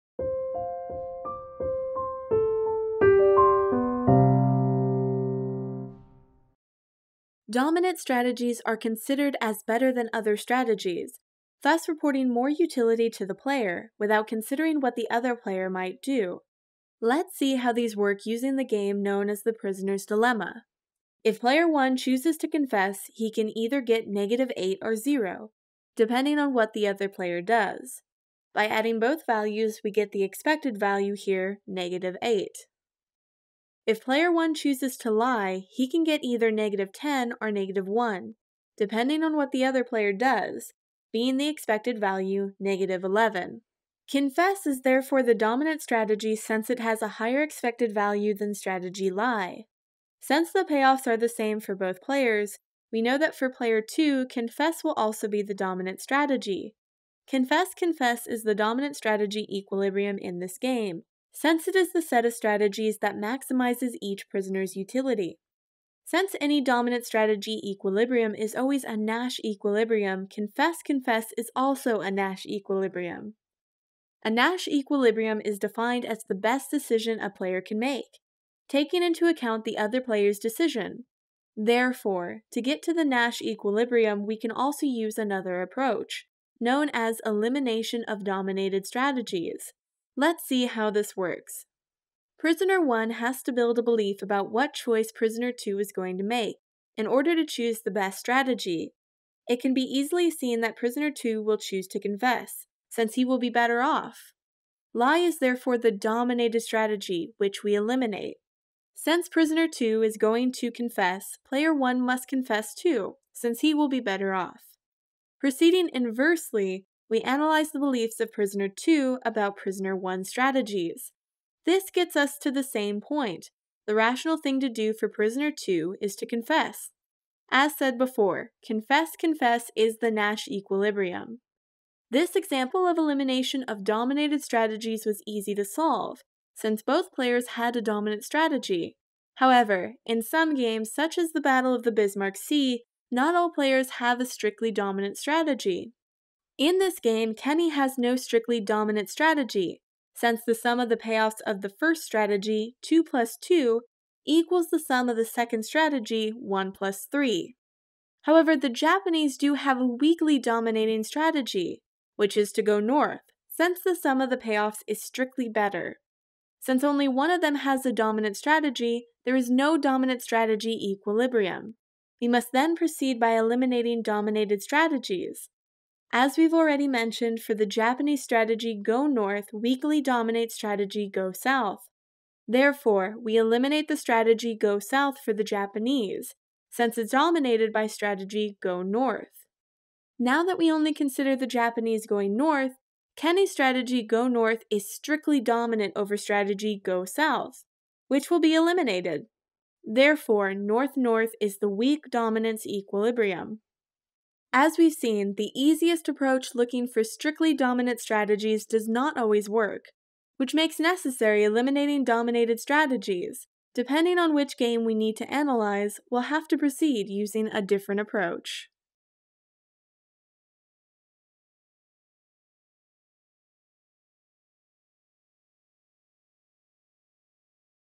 D.5 Dominant strategies and Nash equilibrium | Game Theory - Microeconomics
This video explains how dominant strategies work, and how to reach a Nash equilibrium. We start by analysing dominant strategies, then explain what the Nash equilibrium is. Finally, we show an example of elimination of dominated strategies.